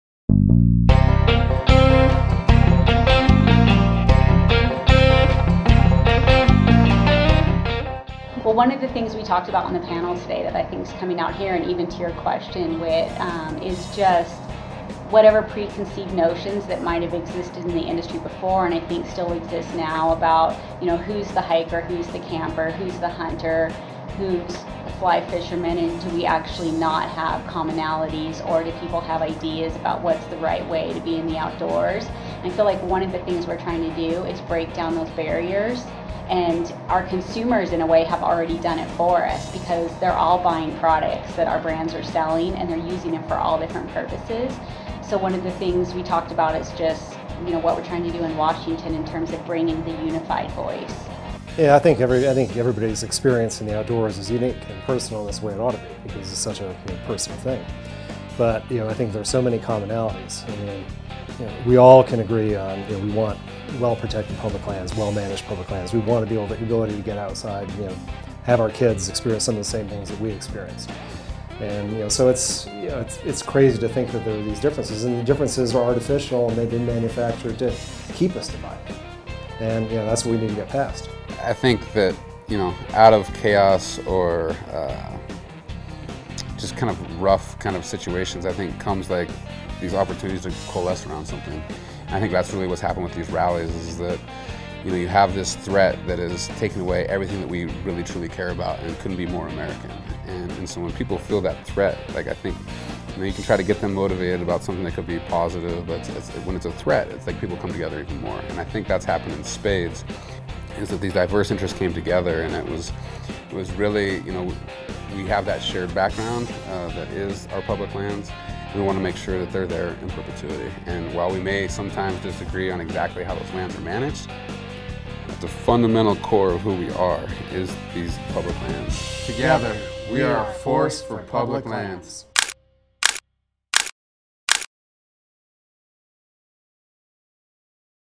Hike, Hook, Bullet
"We have that shared background that is our public lands. We want to make sure they're there into perpetuity."--Land Tawney, CEO & President

Proud to stand with OIA & the Theodore Roosevelt Conservation Partnership on making sure that public lands stay in public hands!